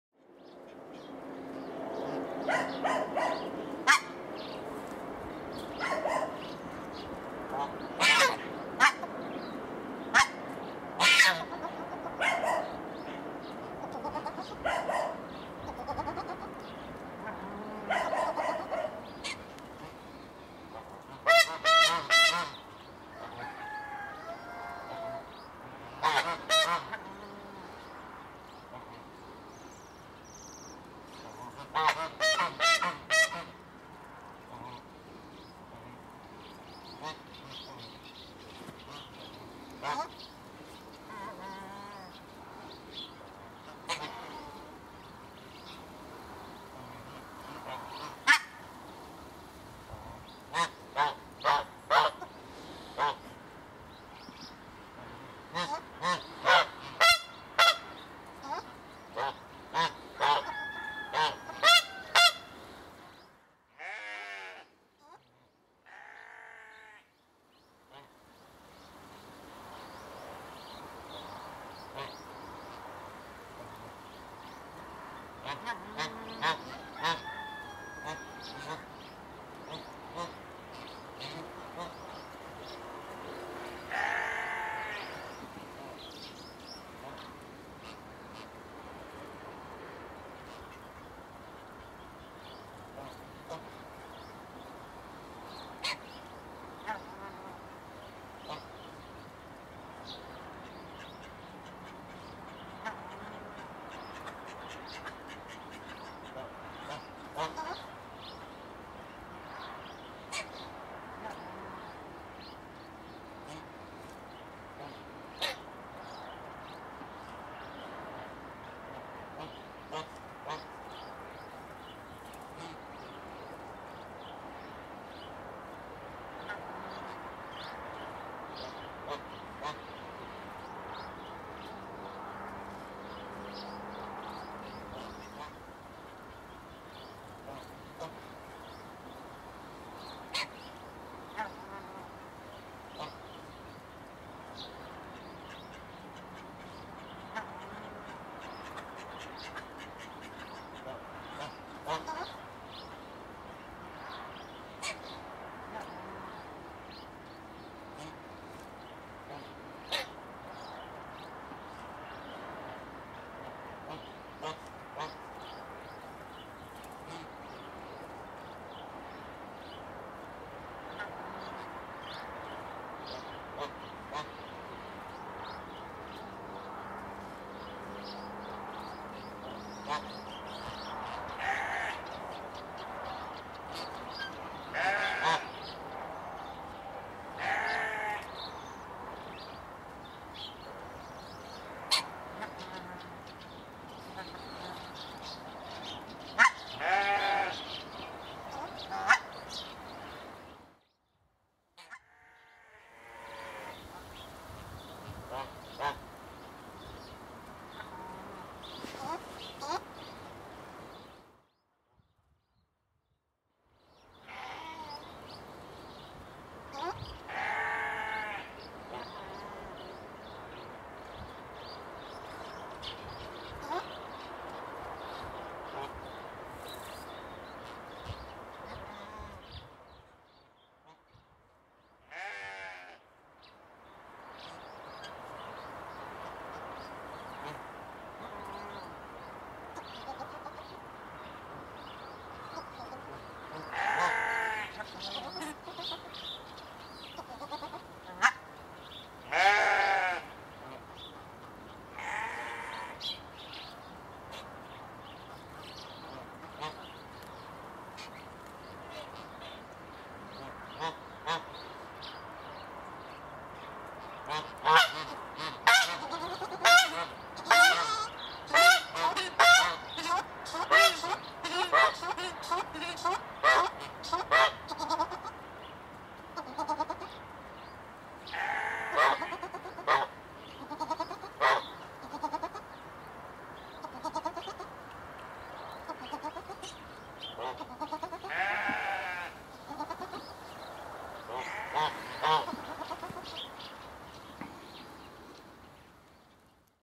Гусь сухонос в открытой вольере (Anser cygnoides)
Пару гусей сухоносов (Anser cygnoides) перевели в открытый вольер.